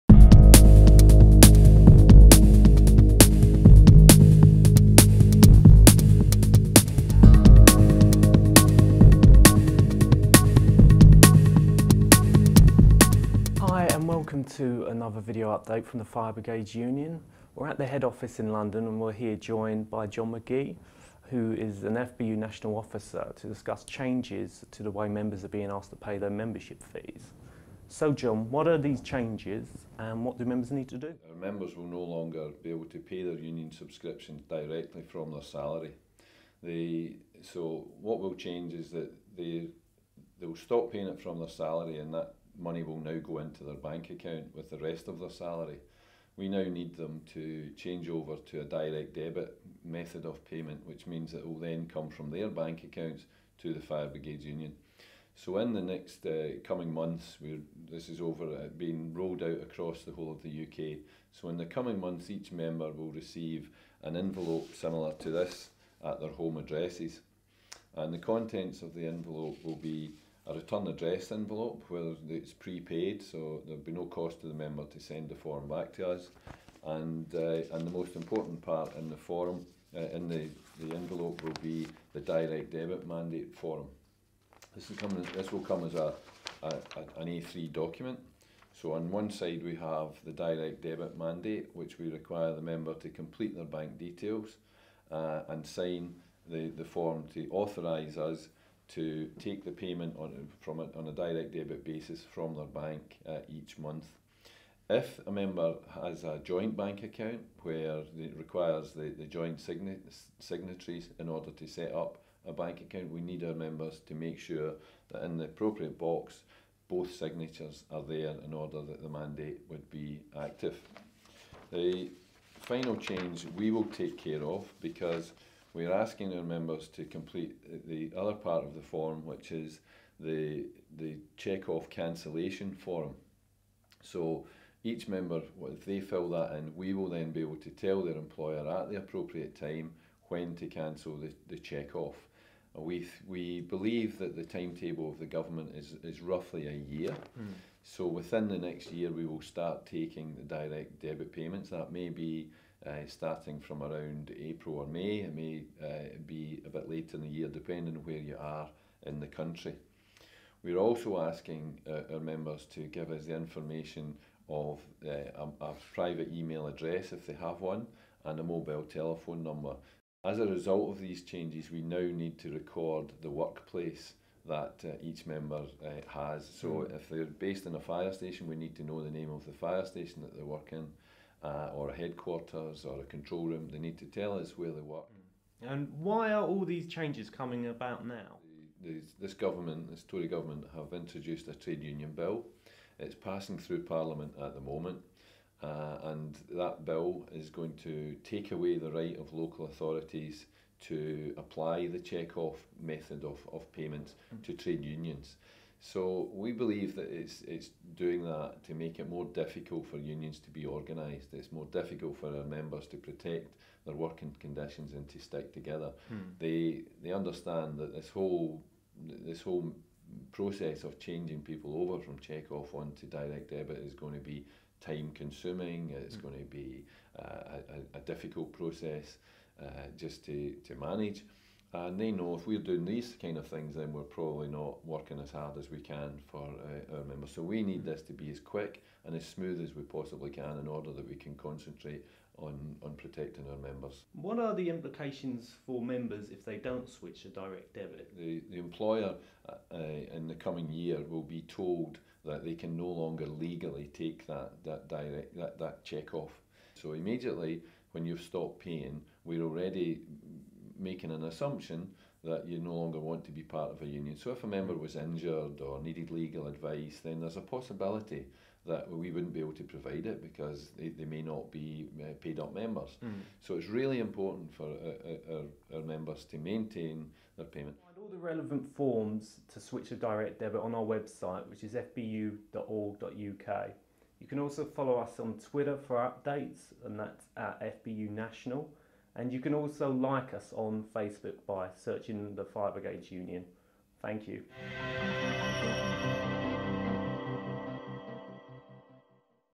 Find out how the FBU is changing how membership fees are paid | Fire Brigades Union
The Fire Brigades Union are changing the way how members pay to be a member of the union. Due to the governments' anti-democratic Trade Union Bill, firefighters and control staff will no longer be able to pay direct from their salary so the union are switching to a direct debit system.

For more info and how you can switch to Direct Debit